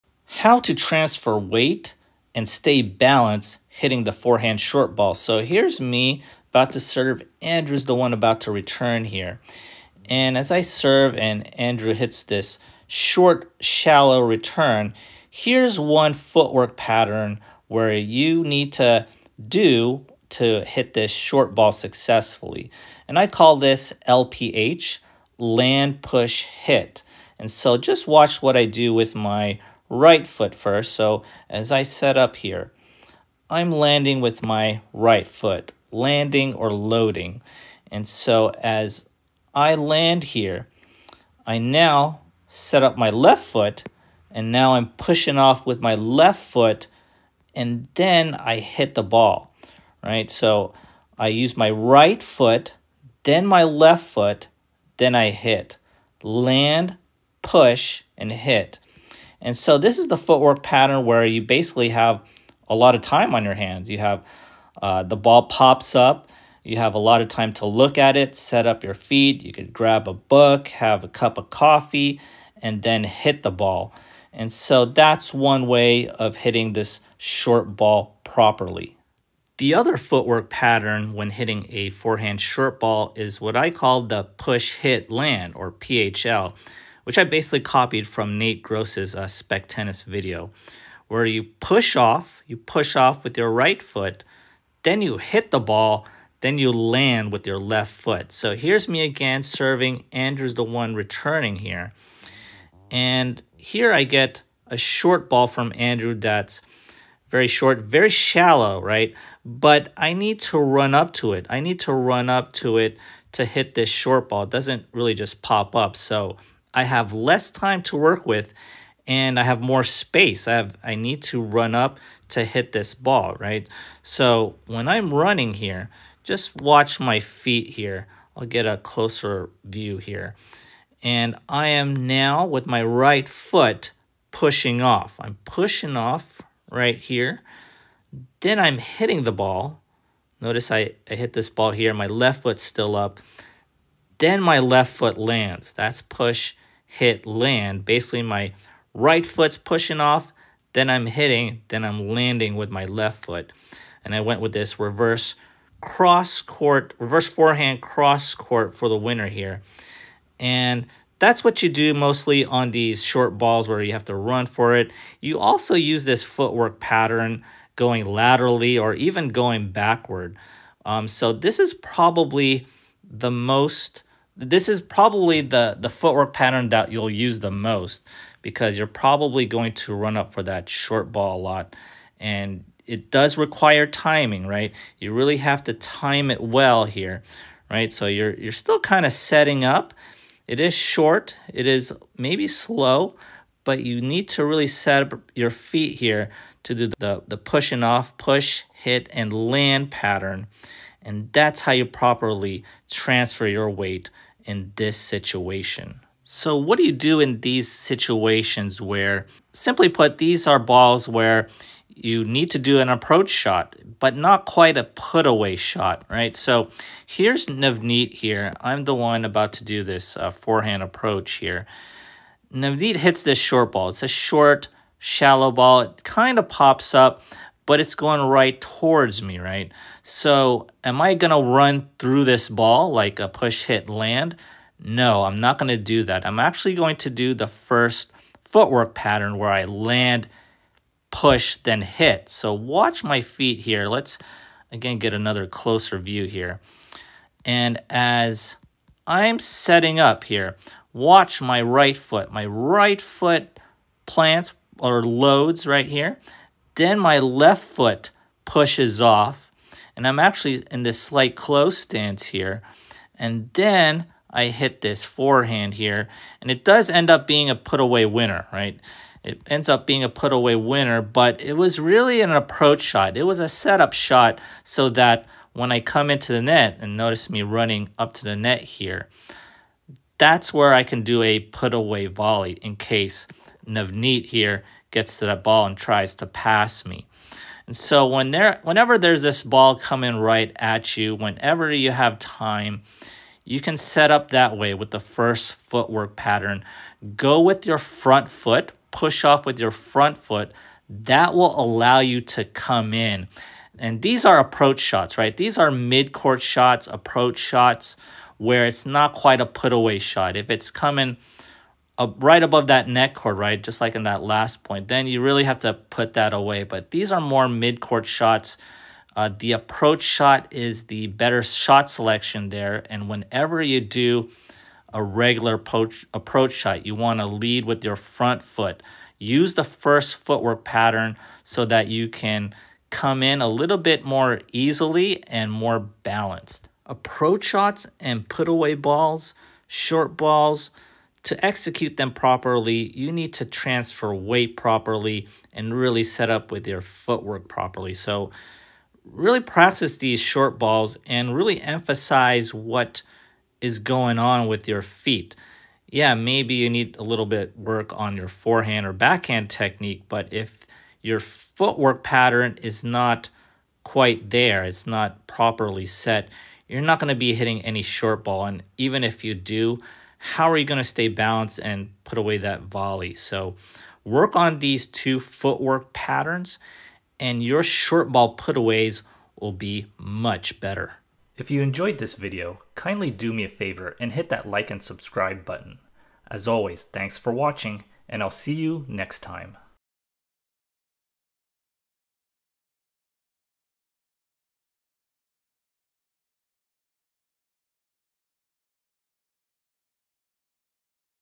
How to Transfer Weight and Stay Balanced on Forehand Short Balls - Tennis Lesson
Coach Adrian shows you how to properly transfer weight and stay balanced hitting a forehand short ball in tennis.

0:00 - Intro
0:22 - LPH (Land, Push, and Hit)
1:19 - PHL (Push, Hit, and Land)
3:26 - Forehand Approach Shots
5:36 - Conclusion

8 WAYS TO BE MENTALLY TOUGH

8 WAYS TO BE MENTALLY TOUGH